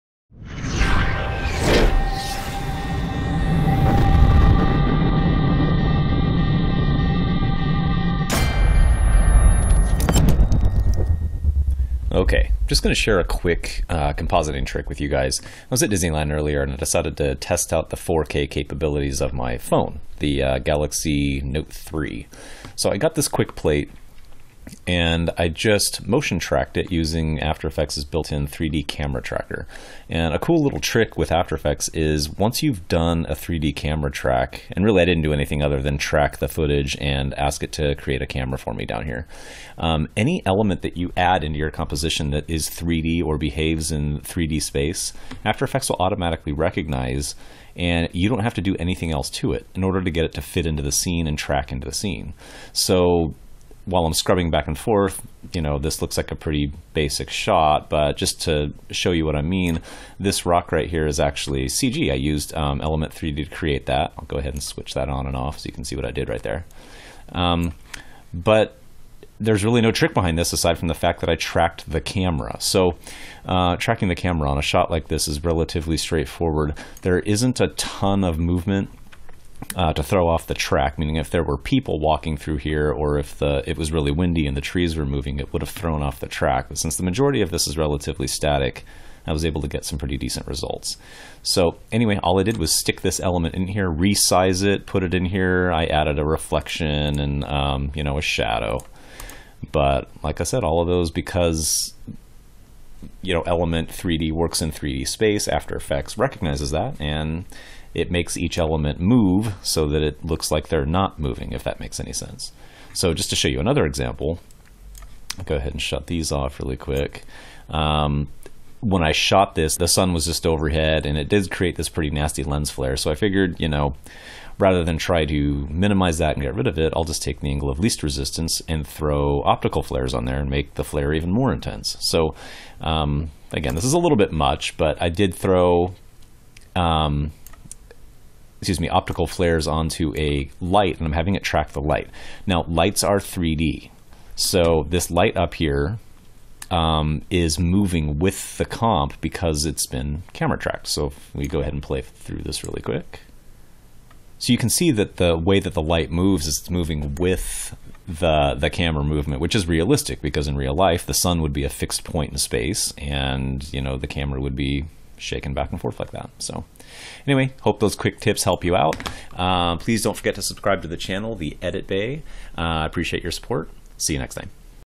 Tips when using After Effects' 3D Camera Tracker
Need to composite multiple elements into a scene? If your footage can be camera tracked, you can easily insert as many items as you'd like and have them "stick" to any position you need them to.